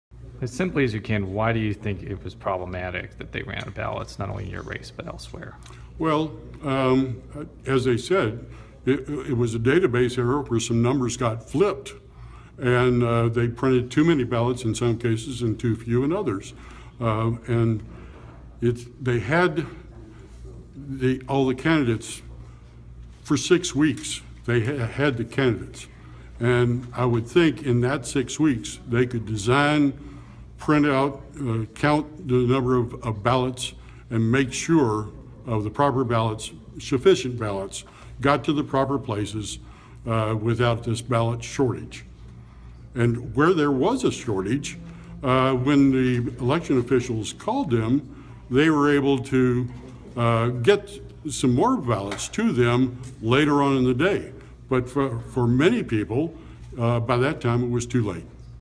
Former Sunset Hills alderman on why the ballot shortage matters
Former Sunset Hills alderman Frank Hardy on why the ballot shortage matters